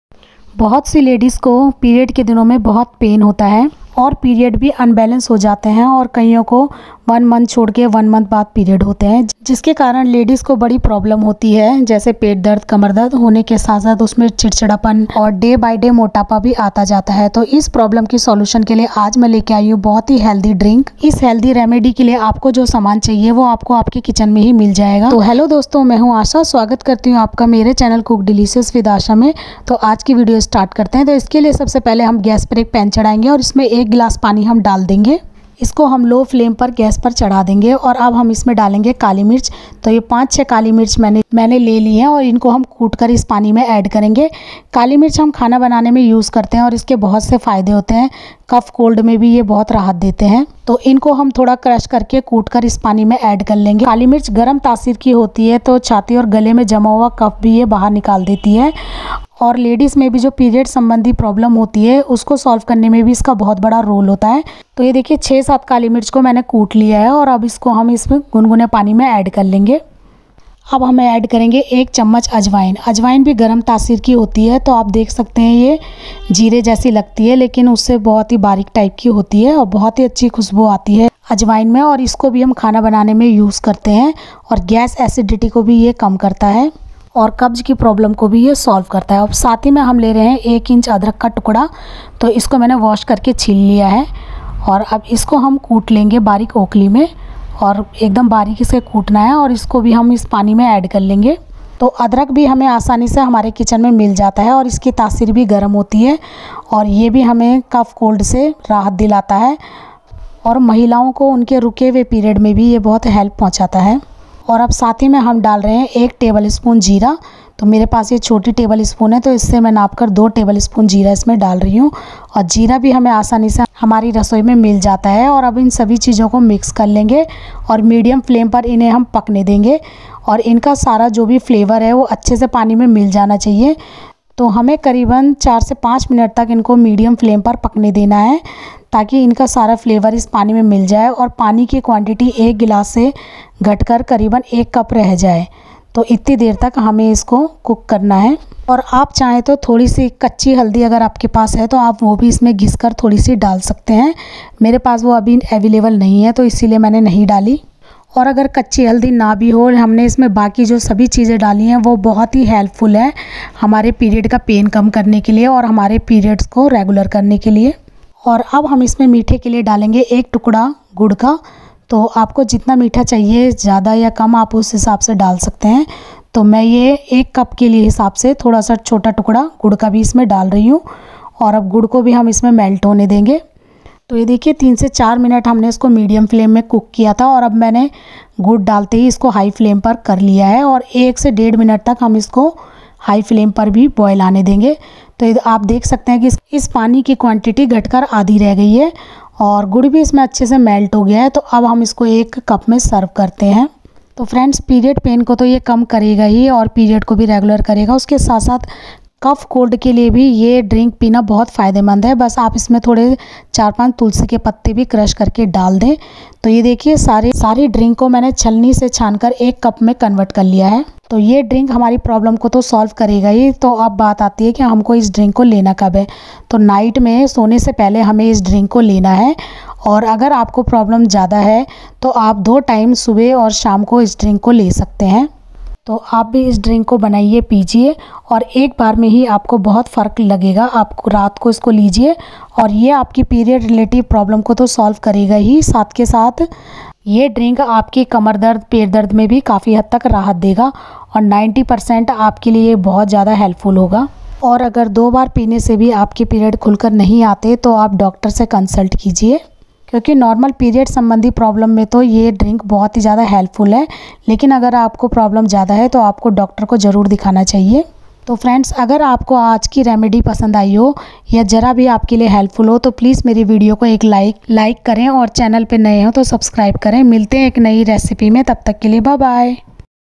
रुका हुआ पीरियड 1 दिनमें ही आ जाएगा | 100% कारगर घरेलू उपाय period pain solution | Irregular periods
रुका हुआ पीरियड 1 दिन में ही आ जाएगा इस 90% परसेंट कारगर घरेलू उपाय से ( remedy for irregular period | periods pain solution)
 आज मैं आपके लिए एक नई रेमेडी लेकर आई हूं  अक्सर महिलाओं में पीरियड संबंधी कई प्रॉब्लम होती हैं । अनियमित महावारी के साथ-साथ  महामारी के समय पेट और कमर में बहुत ज्यादा दर्द होता है । तो इस समस्या के समाधान के लिए हम एक बहुत ही हेल्दी ड्रिंक लेकर आए हैं । इस सामान की सामग्री हमारी रसोई में ही आसानी से मिल जाता है तो प्लीज वीडियो को अंत तक जरूर देखें और अगर आपको मेरी वीडियो पसंद आए तो प्लीज लाइक शेयर सब्सक्राइब करें।